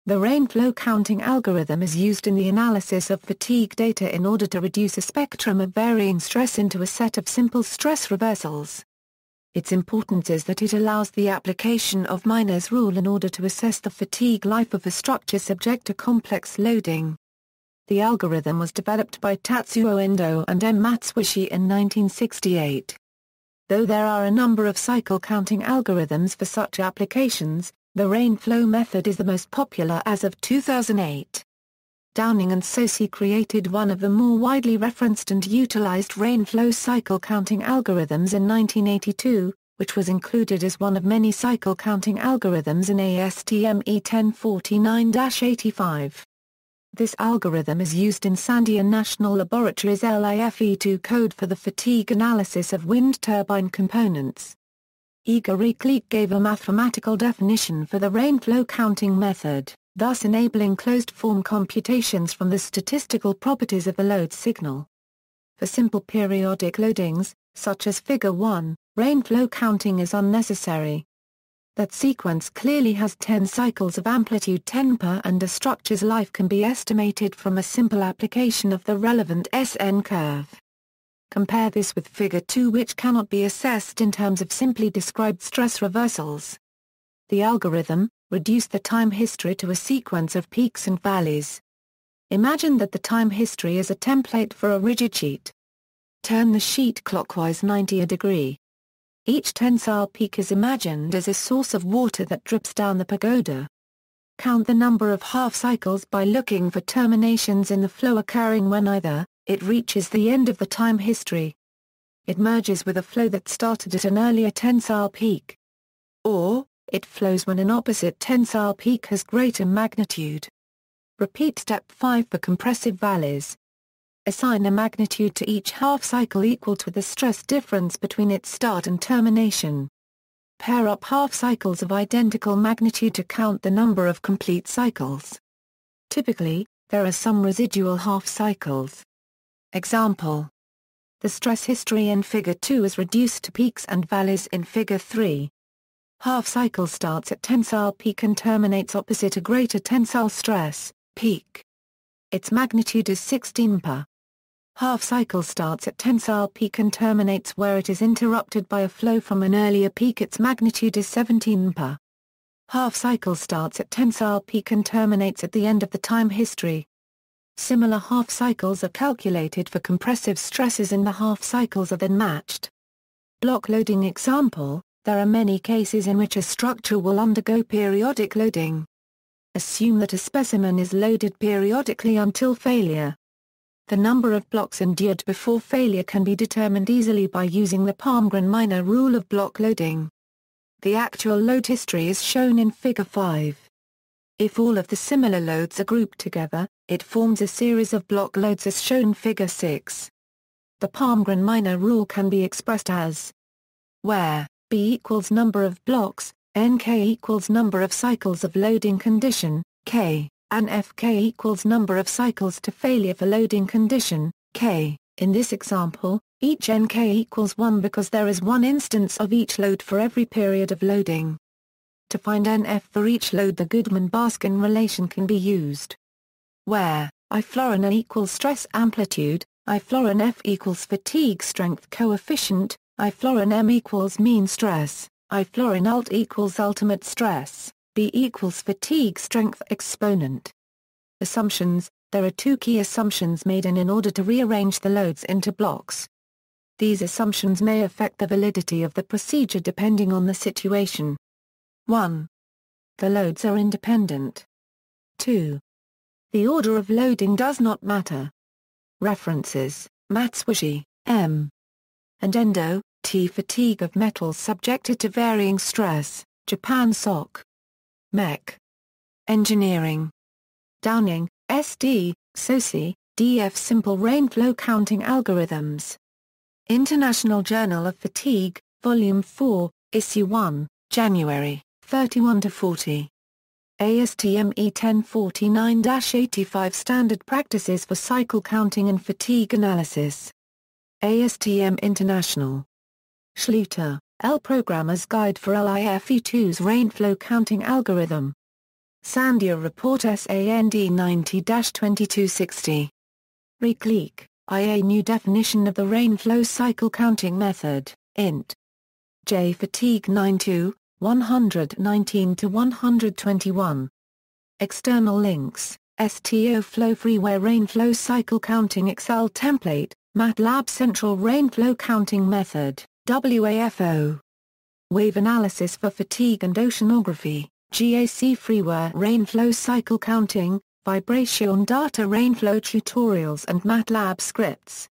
Rainflow-counting algorithm
The rainflow-counting algorithm (also known as the "rain-flow counting method") is used in the analysis of fatigue data in order to reduce a spectrum of varying stress into a set of simple stress reversals. Its importance is that it allows the application of Miner's rule in order to assess the fatigue life of a structure subject to complex loading. The algorithm was developed by Tatsuo Endo and M. Matsuishi in 1968. Though there are a number of cycle-counting algorithms for such applications, the rainflow method is the most popular as of 2008.
Downing and Socie created one of the more widely referenced and utilized rainflow cycle-counting algorithms in 1982, which was included as one of many cycle-counting algorithms in ASTM E 1049-85. This algorithm is used in Sandia National Laboratories LIFE2 code for the fatigue analysis of wind turbine components.